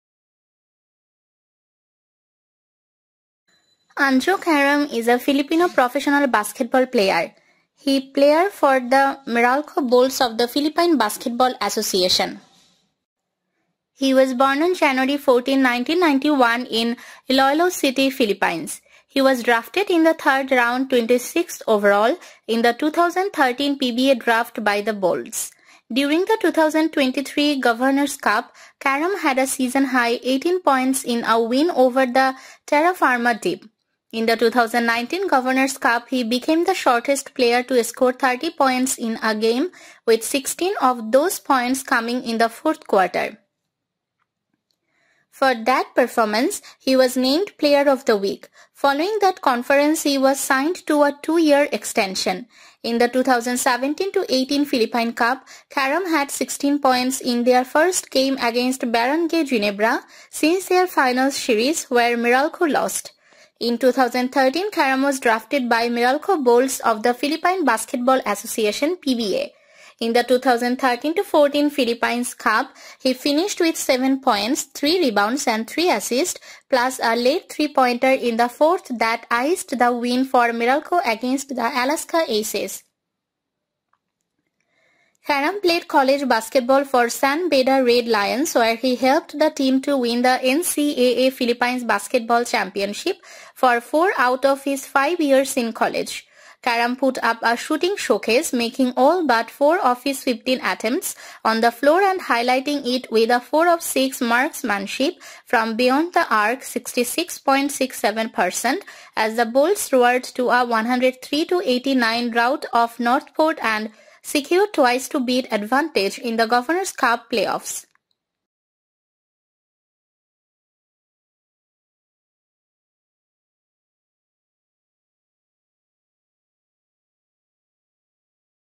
Anjo Caram (Basketball Player) || 5 Things You Didn't Know About Anjo Caram
Anjo Caram  is a Filipino professional basketball player .
He player for the Meralco Bolts of the Philippine Basketball Association.
He was Born on January 14, 1991 in Iloilo City, Philippines.
He was drafted in the third round (26th overall) in the 2013 PBA draft by the Bolts.
During the 2023 Governors' Cup, Caram had a season-high 18 points in a win over the Terrafirma Dyip.